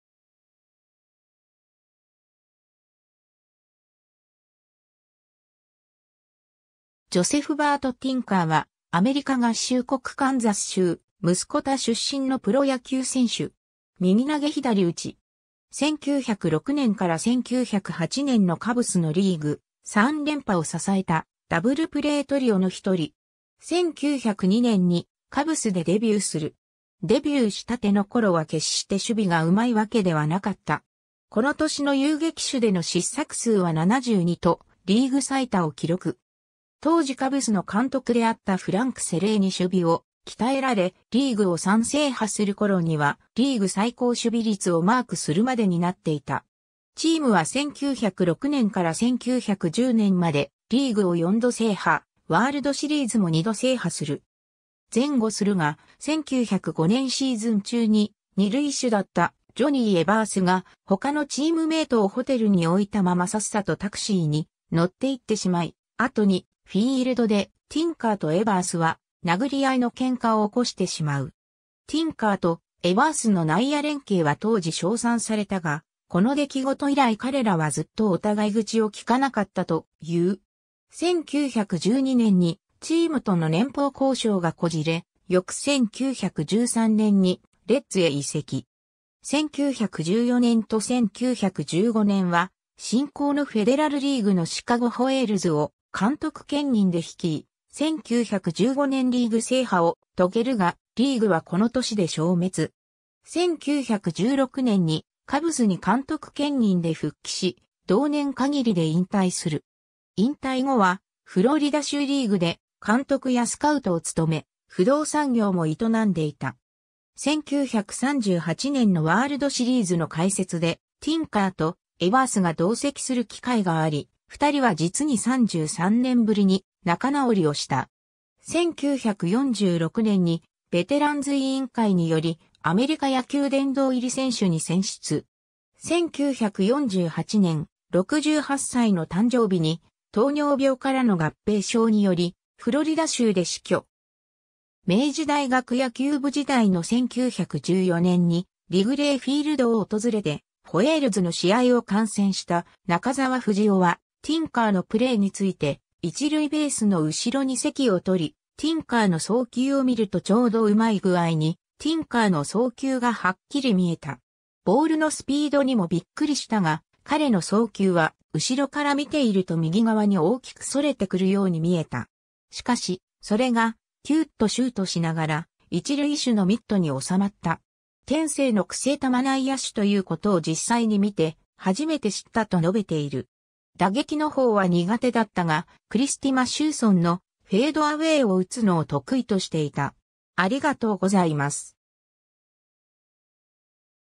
ジョー・ティンカー
ジョセフ・バート・ティンカー（Joseph Bert Tinker, 1880年7月27日 - 1948年7月27日）は、アメリカ合衆国カンザス州ムスコタ出身のプロ野球選手（遊撃手）。
1906年から1908年のカブスのリーグ3連覇を支えた「ダブルプレー・トリオ」の一人。
 1902年にカブスでデビューする。
デビューしたての頃は決して守備が上手いわけではなかった。
この年の遊撃手での失策数は72とリーグ最多を記録。
当時カブスの監督であったフランク・セレーに守備を鍛えられ、リーグを3制覇する頃には、リーグ最高守備率をマークするまでになっていた。
チームは1906年から1910年までリーグを4度制覇、ワールドシリーズも2度制覇する。
 前後するが、1905年シーズン中に、二塁手だったジョニー・エバースが他のチームメイトをホテルに置いたままさっさとタクシーに乗って行ってしまい、後にフィールドでティンカーとエバースは殴り合いのケンカを起こしてしまう。
ティンカーとエバースの内野連携は当時賞賛されたが、この出来事以来彼らはずっとお互い口をきかなかったという。
 1912年にチームとの年俸交渉がこじれ、翌1913年にレッズへ移籍。
 1914年と1915年は新興のフェデラル・リーグのシカゴ・ホエールズを監督兼任で率い、1915年リーグ制覇を遂げるが、リーグはこの年で消滅。
 1916年にカブスに監督兼任で復帰し、同年限りで引退する。
 引退後はフロリダ州リーグで監督やスカウトを務め、不動産業も営んでいた。
1938年のワールドシリーズの解説でティンカーとエバースが同席する機会があり、2人は実に33年ぶりに仲直りをした。
 1946年に、ベテランズ委員会によりアメリカ野球殿堂入り選手に選出。
1948年、68歳の誕生日に糖尿病からの合併症により、フロリダ州で死去。
 明治大学野球部時代の1914年にリグレー・フィールドを訪れてホエールズの試合を観戦した中澤不二雄はティンカーのプレーについて「一塁ベースの後ろに席を取り、ティンカーの送球を見るとちょうど上手い具合にティンカーの送球がはっきり見えた。ボールのスピードにもビックリしたが、彼の送球は後ろから見ていると右側に大きく逸れてくるように見えた。しかし、それがキューッとシュートしながら、一塁手のミットに収まった。天性のクセ球内野手という事を実際に見て初めて知った」と述べている。
打撃の方は苦手だったが、クリスティ・マシューソンの「フェイド・アウェイ」（今で言うスクリューボール）を打つのを得意としていた。